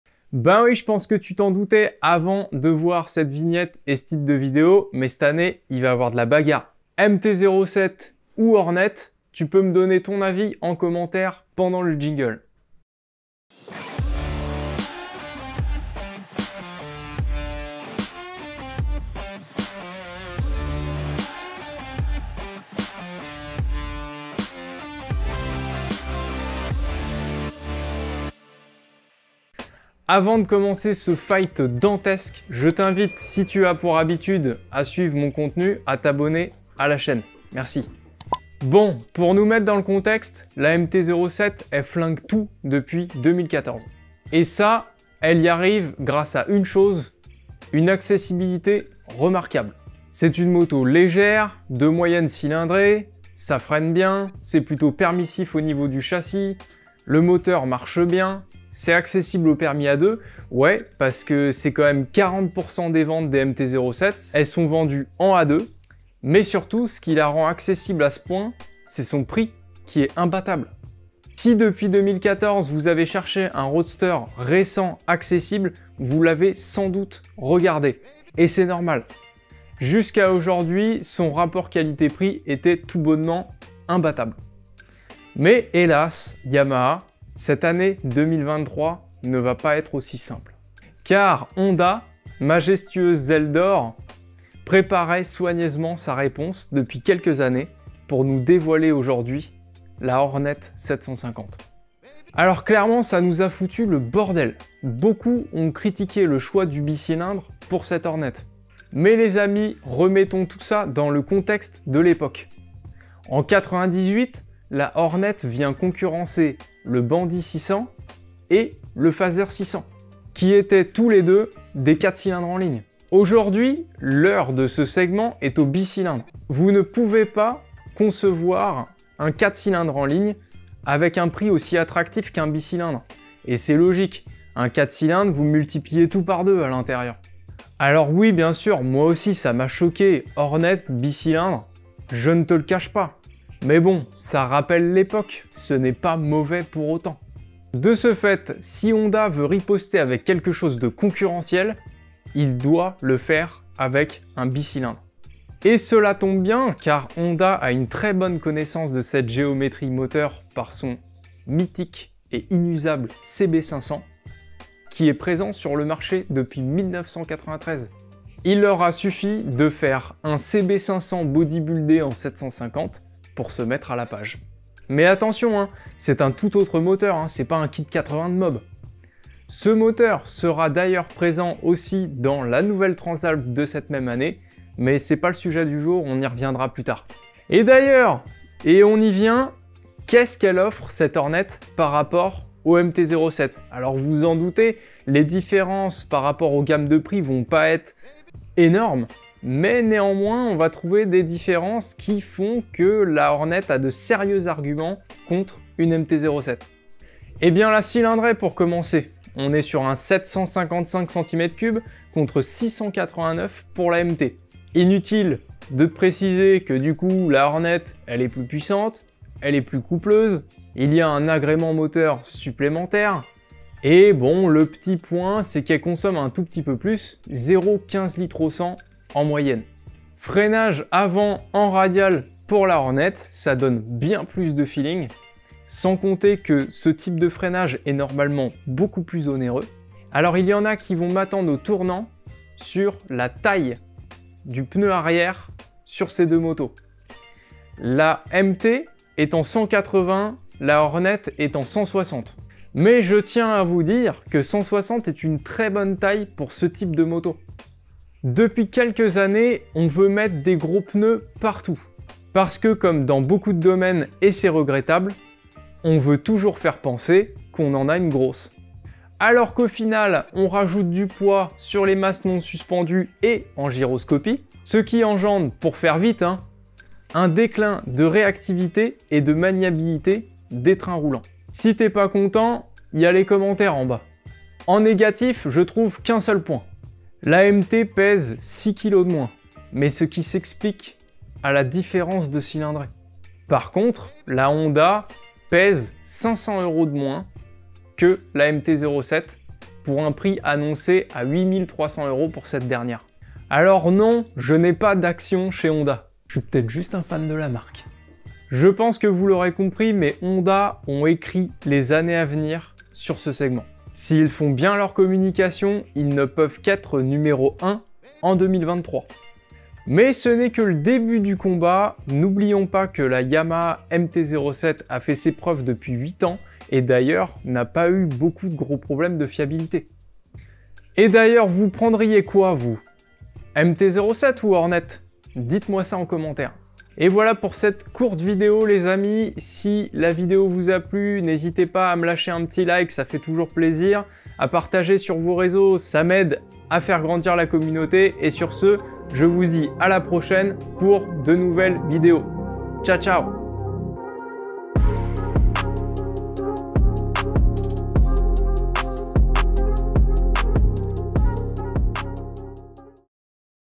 YAMAHA MT-07 vs HONDA HORNET 750, un combat dantesque cet hiver ?
Et voilà 2023, grande année de changement sur le segment des roadsters.
Les cartes sont rebalayées, Honda prend d'assaut l'indétrônable Yamaha MT-07.

Vous avez un pronostique ?